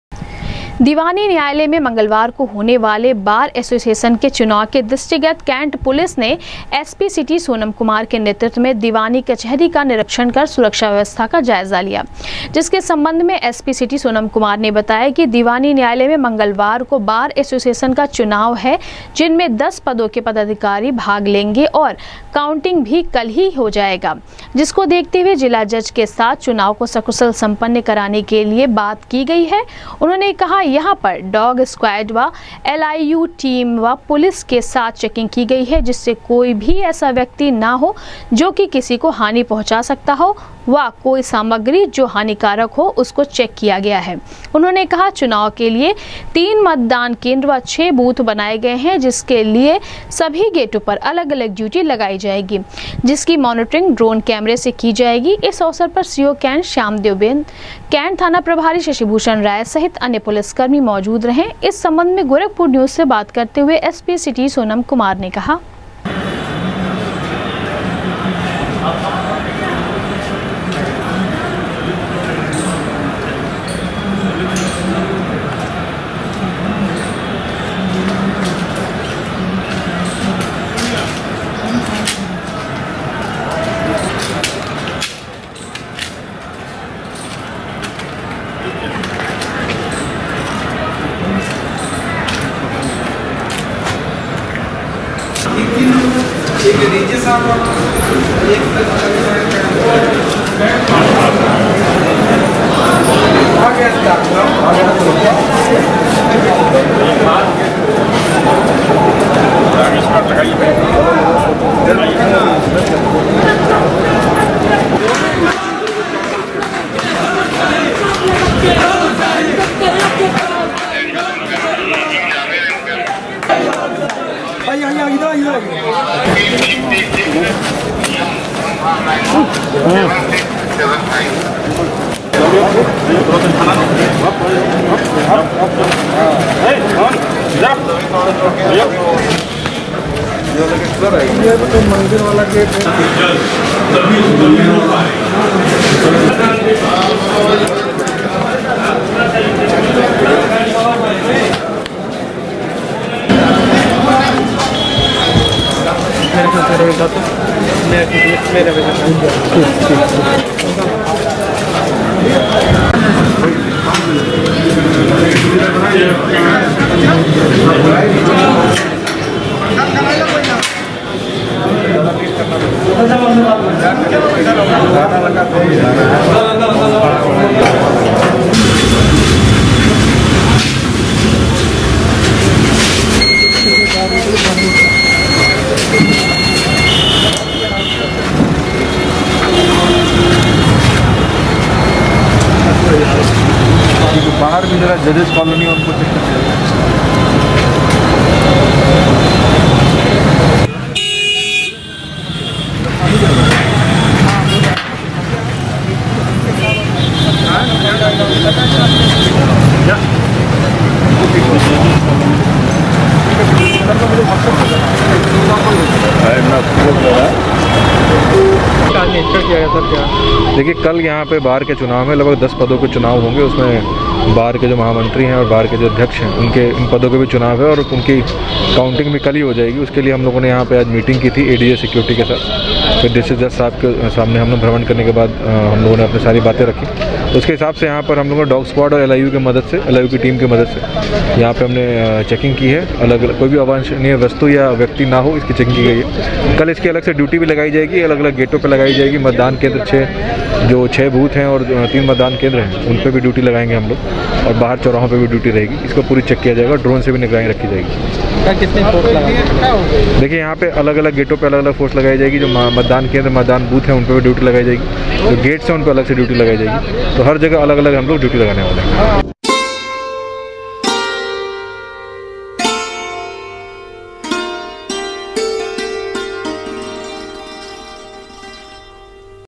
मंगलवार को बार एसोसिएशन के होने वाले चुनाव के दृष्टिगत कैंट पुलिस ने एसपी सिटी सोनम कुमार के नेतृत्व
मंगलवार को बार एसोसिएशन के होने वाले चुनाव के दृष्टिगत कैंट पुलिस ने एसपी सिटी सोनम कुमार के नेतृत्व में दीवानी परिसर के सुरक्षा व्यवस्था का लिया जायजा,कहा, चुनाव वाले दिन दीवानी में अलग से पुलिस कर्मियों की लगायी जायेगी ड्यूटी,सकुशल सम्पन्न करवाया जायेगा चुनाव